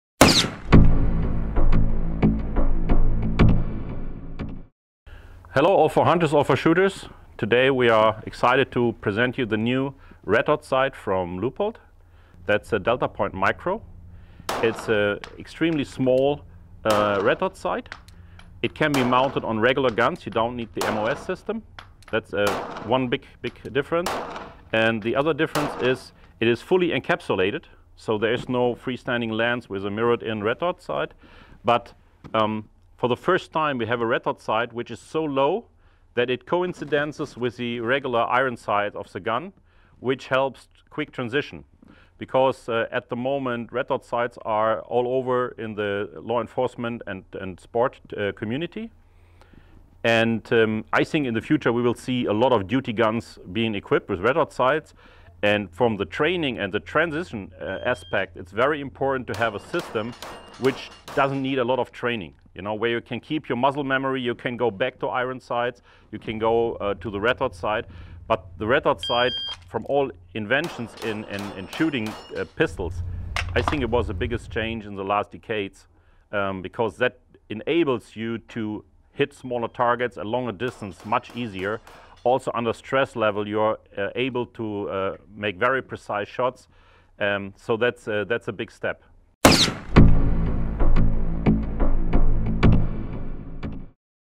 9-time German Champion Marijan Loch tests the Leupold DeltaPoint Micro red dot (part 1)
There is not much more to say about Marijan Loch. 9 German championships, 5 titles at the "European Steel Challenge" and 60 other international competitions won speak volumes. Marijan Loch is someone whose opinion has a qualified value in the industry and is not measured just by followers and clicks. Loch tested for us the new Leupold DeltaPoint Micro red dot – Watch the video.

DeltaPoint Micro from Leupold: a compact design
The huge advantage of the DeltaPoint Micro is certainly its compact design, so when used with handguns (we mounted the DPM on a GLOCK 19 and a Walther pistol) you will feel a huge advantage in combination with holsters. We tested the red dot sight-handgun combination in 4 different holsters – with no problems. The following holsters were used: Blackhawk Level 1, Blackhawk T-Series L2C, Kydex Sportholster and the "Omnivore" holster from Blackhawk which can accept up to 150 different handguns models.
The DeltaPoint Micro aligns the red dot with the front sight and is mounted at the same height as the factory iron sights. This makes target acquisition intuitive and accurate. The enclosed system of the DeltaPoint Micro from Leupold is also designed for tough conditions. In the video below, Jens Tigges from Outdoor Marketing explains the features of the DeltaPoint Micro.